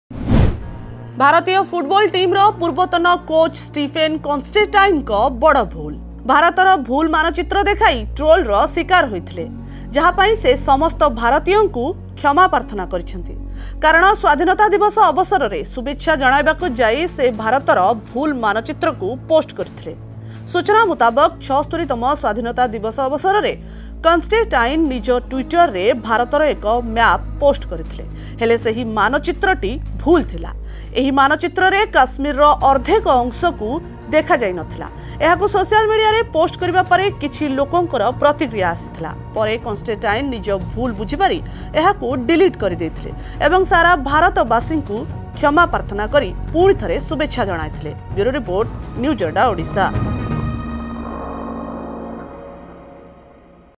Former Football Coach Lands In Controversy For Sharing Wrong India Map In Social Media, Apologized!
ଭାରତୀୟ ଫୁଟବଲ ଟିମ୍‌ର ପୂର୍ବତନ କୋଚ୍ ଷ୍ଟିଫେନ୍ କନଷ୍ଟେଟାଇନ୍‌ଙ୍କ ବଡ ଭୂଲ । ଭାରତର ଭୂଲ ମାନଚିତ୍ର ଦେଖାଇ  ଟ୍ରୋଲର ଶୀକାର ହୋଇଥିଲେ । ଯାହା ପାଇଁ ସେ ସମସ୍ତ ଭାରତୀୟଙ୍କୁ କ୍ଷମା ପ୍ରାର୍ଥନା କରିଛନ୍ତି । କାରଣ ସ୍ୱାଧୀନତା ଦିବସ ଅବସରରେ ଶୁଭେଚ୍ଛା ଜଣାଇବାକୁ ଯାଇ ସେ  ଭାରତର ଭୂଲ ମାନଚିତ୍ରକୁ ପୋଷ୍ଟ କରିଥିଲେ ।